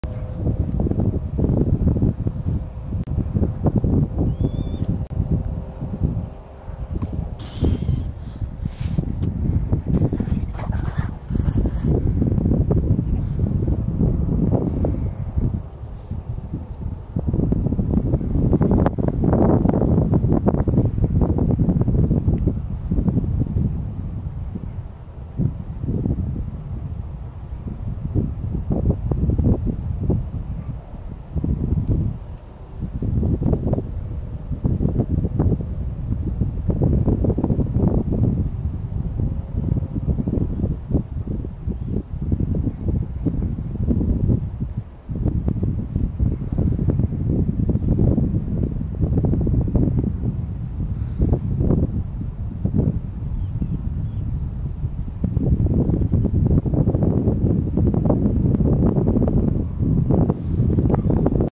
Deer with young / Reegeit met jong
filmed in the nature reserve de Polders van Kruibeke